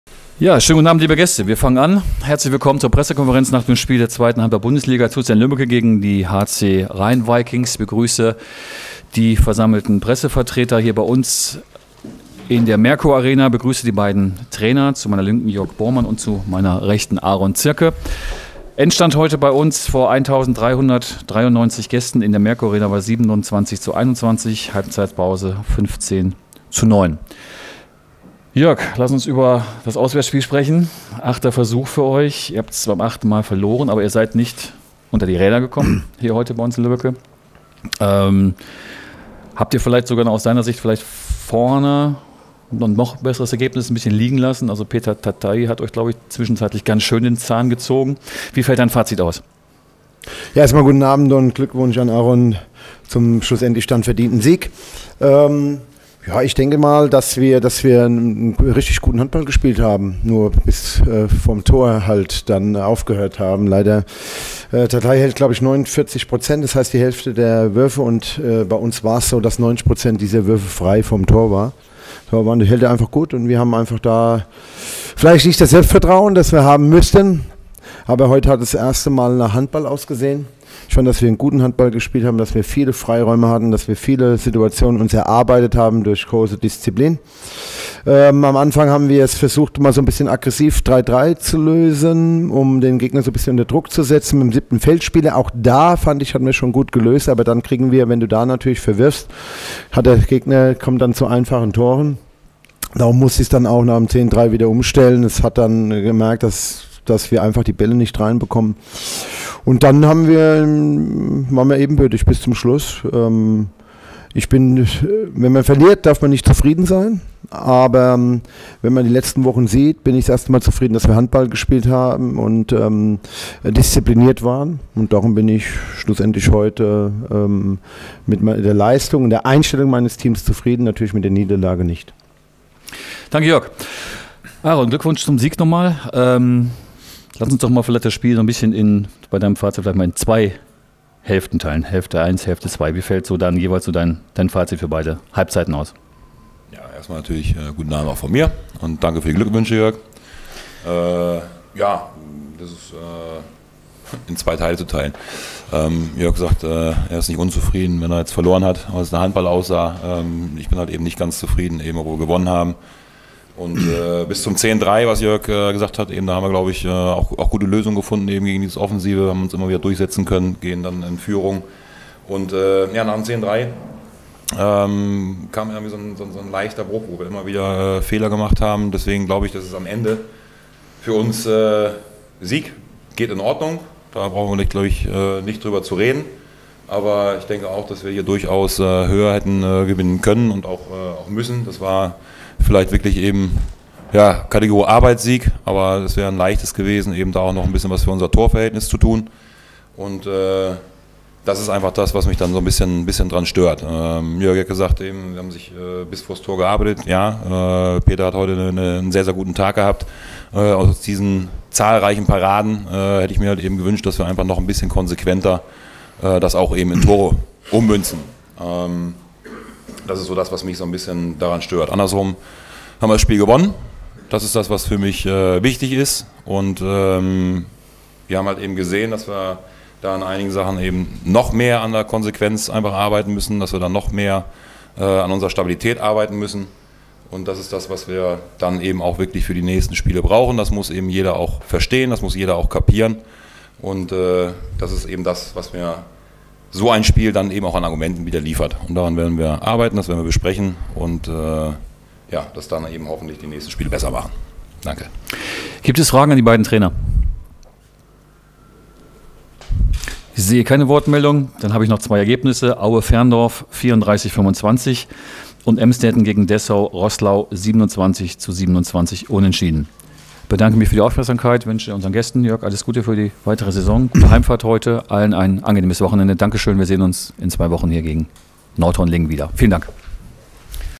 TuS N-LÜbbecke vs. HC Rhein Vikings
Die Pressekonferenz nach dem 27:21-Sieg des TuS N-Lübbecke über den HC Rhein Vikings mt den Trainern Jörg Bohrmann (Vikings) und Aaron Ziercke (TuS).